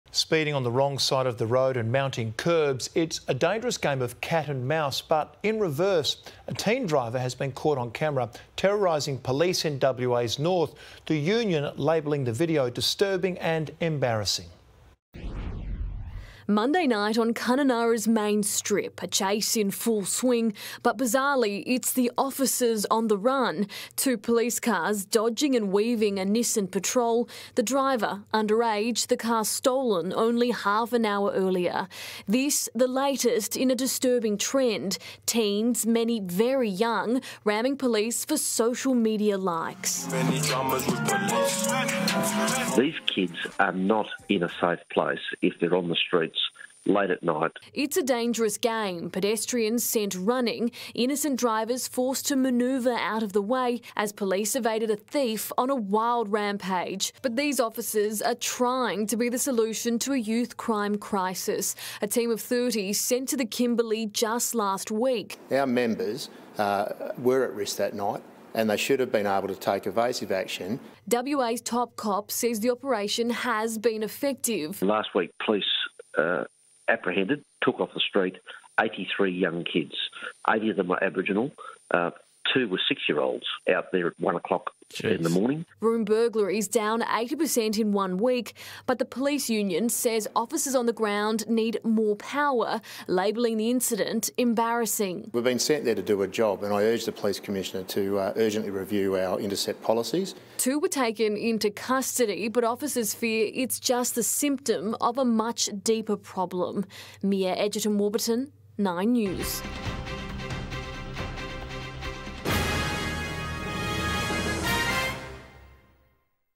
Speeding on the wrong side of the road and mounting kerbs, it's a dangerous game of cat and mouse
Speeding on the wrong side of the road and mounting kerbs, it's a dangerous game of cat and mouse... but in reverse.

A teen driver has been caught on camera terrorising police in WA's north, the Union labelling the video - "disturbing and embarrassing".

9News | Nightly at 6.00pm